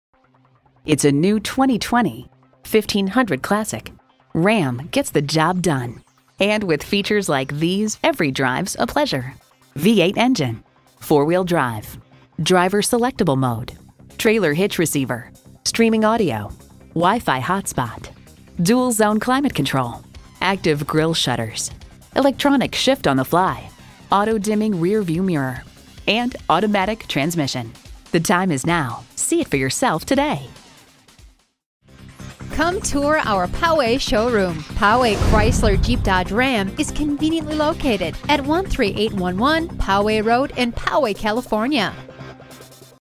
New 2020 Ram 1500 Classic Poway CA Escondido, CA
Year: 2020
Make: Ram
Model: 1500 Classic
Trim: Tradesman
Engine: HEMI 5.7L V8 Multi Displacement VVT
Transmission: 8-Speed Automatic
Color: White
Interior: Black
Stock #: C18036
VIN: 3C6RR7KT8LG290458
Bright White Clearcoat 2020 Ram 1500 Classic Tradesman 4WD 8-Speed Automatic HEMI 5.7L V8 Multi Displacement VVT

Hours